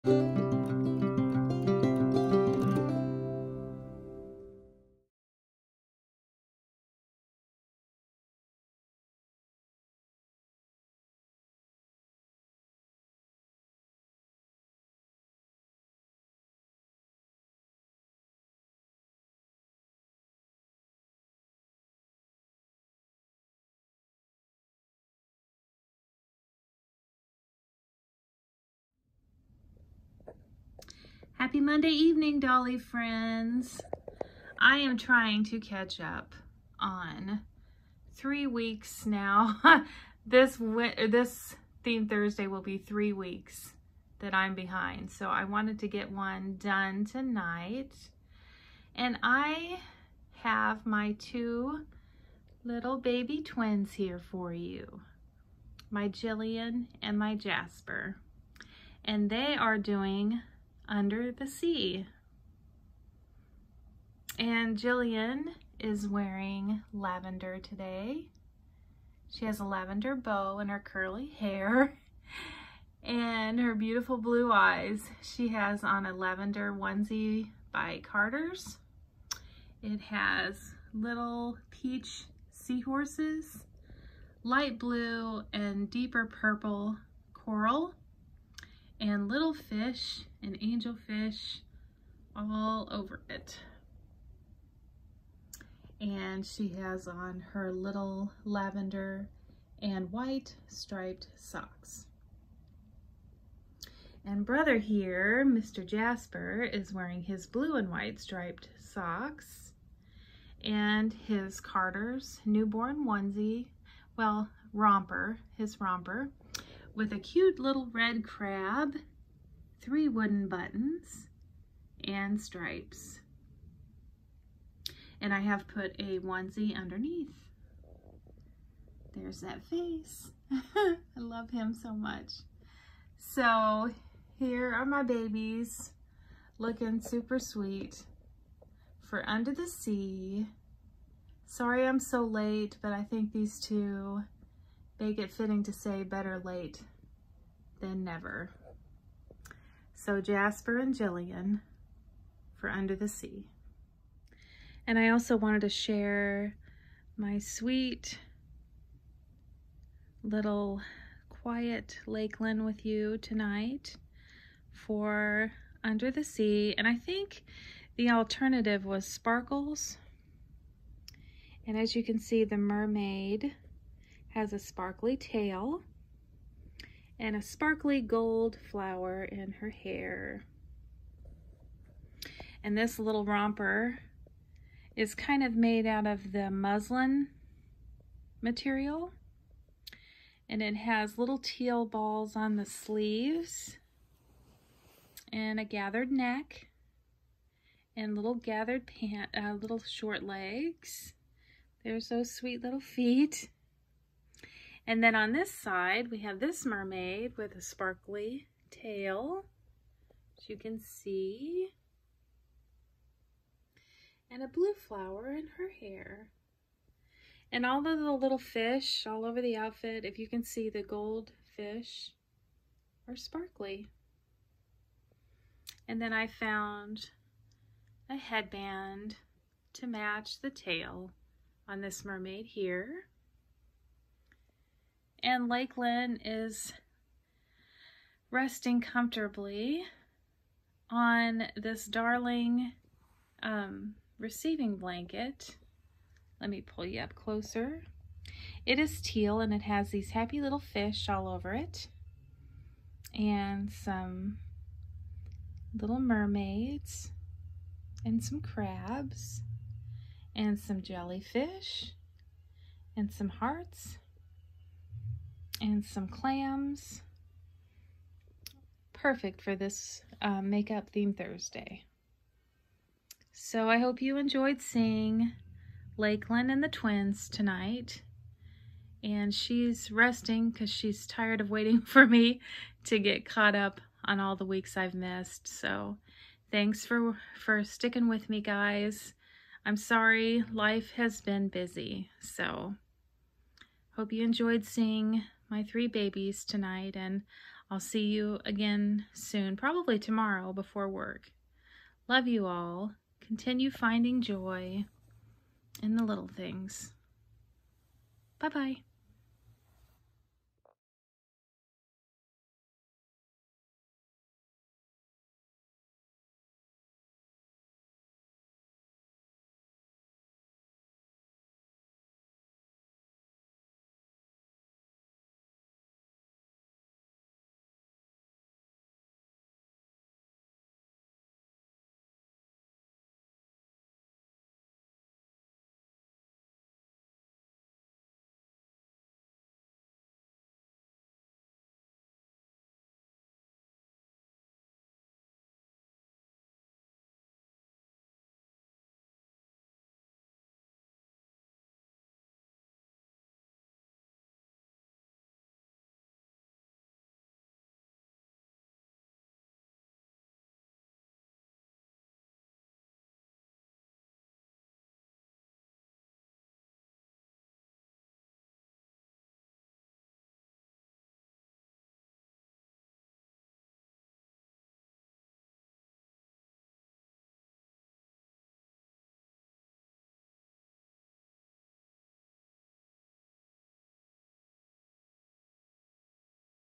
Theme Thursday! Under the Sea 💛🐠💜🧜‍♀️💙
Jillian, Jasper & Lakelyn are helping me catch up on the Theme Thursdays we have missed so far.  I hope you enjoy seeing them all dressed up Under the Sea!  Lots of love, Janae